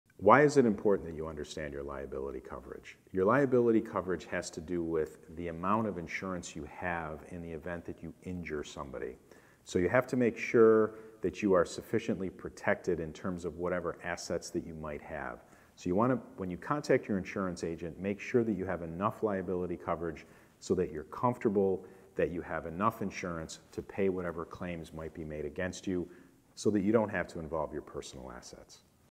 Why It's Important To Understand Your Liability Coverage | Cellino Law | Personal Injury Attorney
Auto accidents can be life-changing. That's why it's important to understand the liability coverage available to you.

Cellino Law, LLP
Buffalo | Rochester | NYC | Long Island | Bridgeport, CT